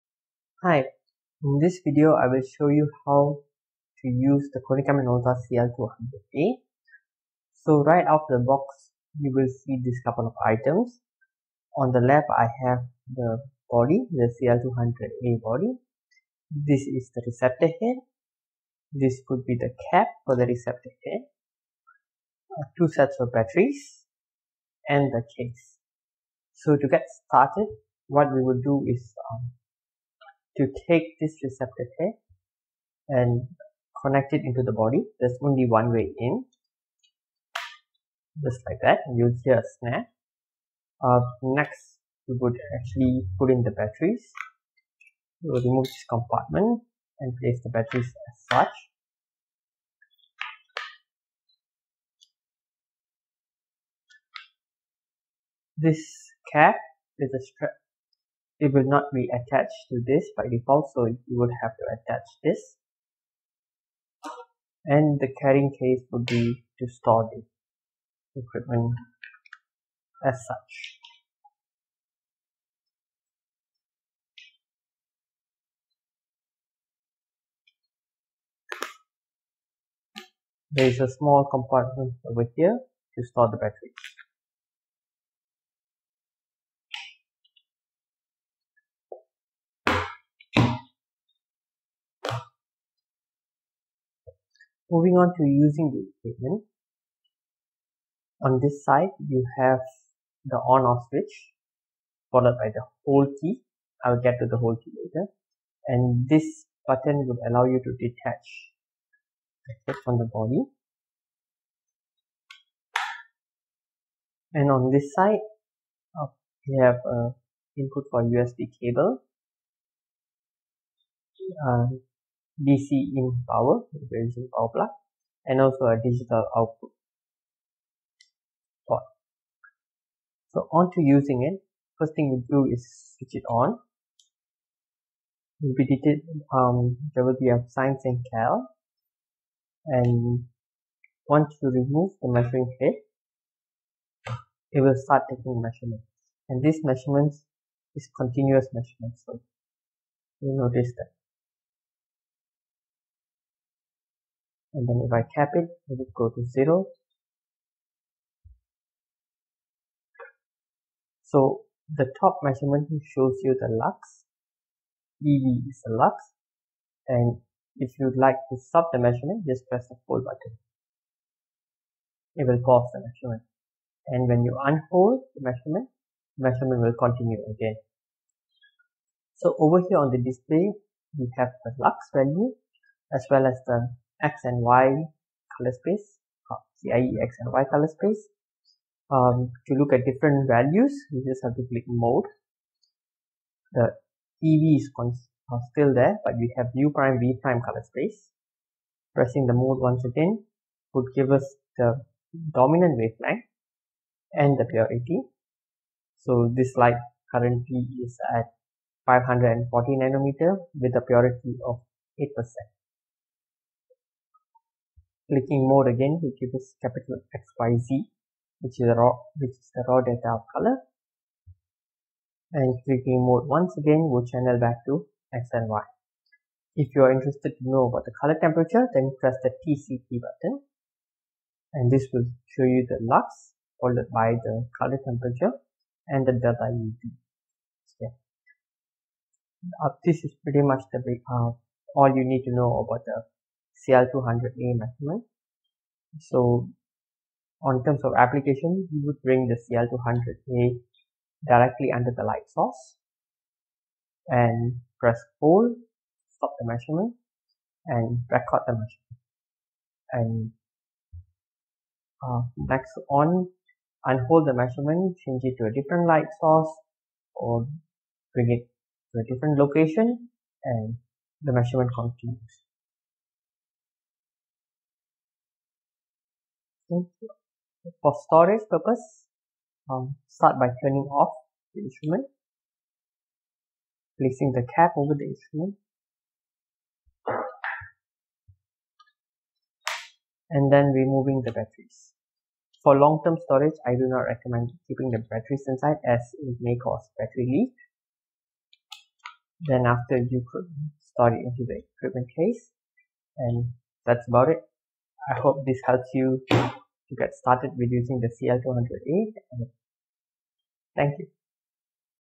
Konica Minolta T-10A & CL-200A User Guide
🌟 Konica Minolta T-10A & CL-200A Training Video 🌟

Welcome to our comprehensive training video on the Konica Minolta CL-200A & T-10A ! Whether you're a beginner or an experienced user, this tutorial is designed to help you master the features and functions of the CL-200A & T-10A.

📹 Video Highlights:
- Intro: 00:00
- Item Description: 00:07
- Usage: 02:10

🔍 Topics Covered:
1. **Introduction to CL-200A**: Get to know the basics and the purpose of this powerful device.
2. **Key Features Overview**: Explore the standout features that make the CL-200A a must-have tool.
3. **Step-by-Step Operation**: Learn how to use the device efficiently with a detailed walkthrough.